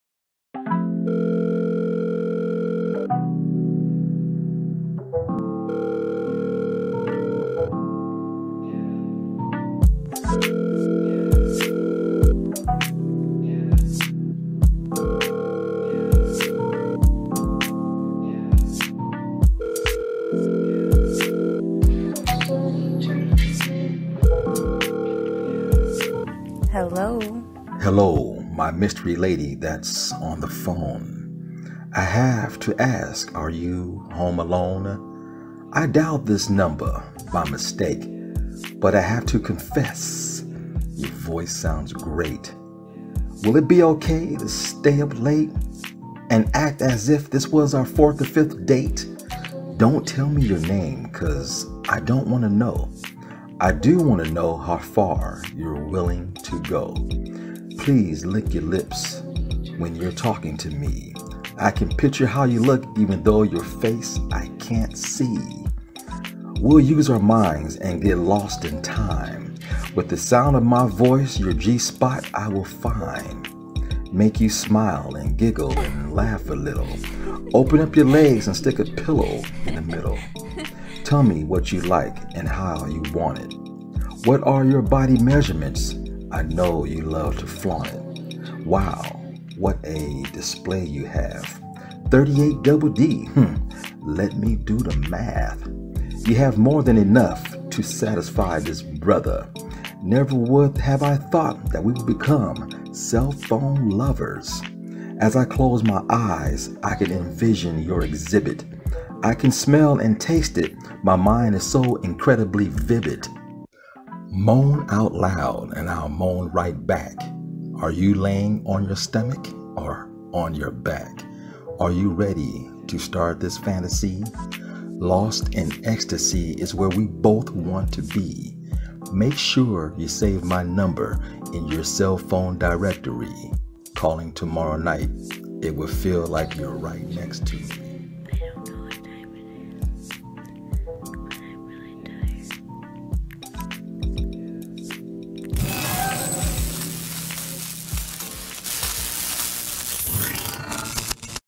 PHONE SEX
Imagine calling the wrong number and starting a relationship over the phone.